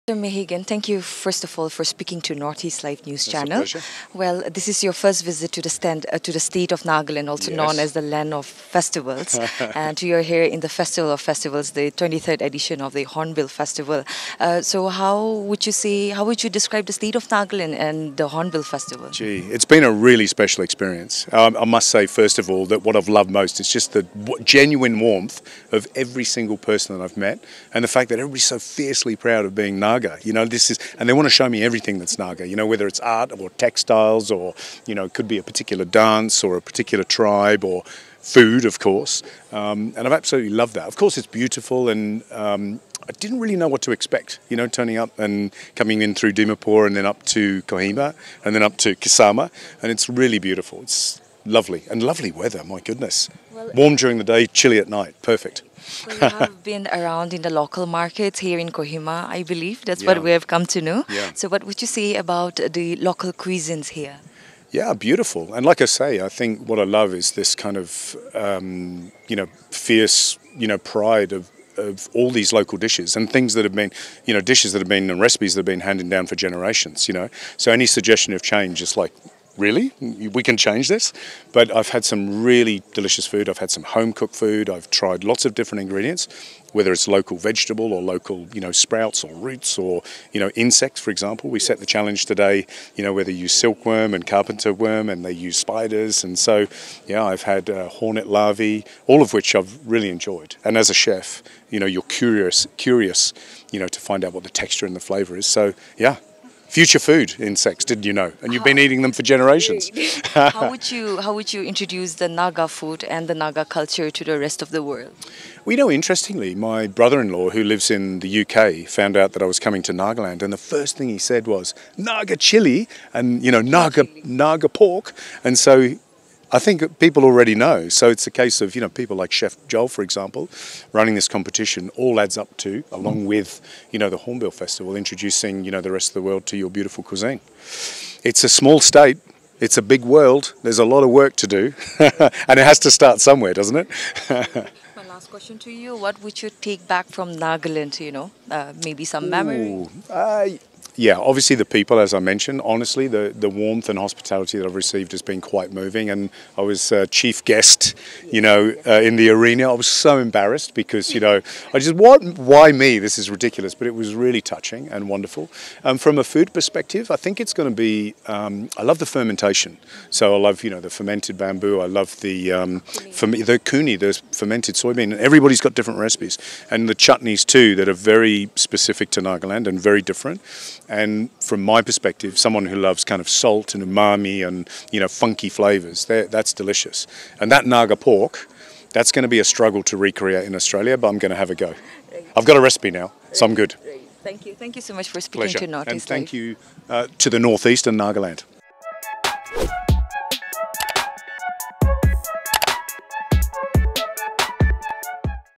'Master Chef' judge Gary Mehigan admires taste of Naga Chilli & Naga Pork
9th Season of Naga Chef in Hornbill Festival: Australian celebrity Chef Gary Mehigan speaks to Northeast Live. Admires taste of Naga chilli and Naga Pork besides other local cuisines. Mehigan expresses gratitude to people of Nagaland for their warmth and hospitality.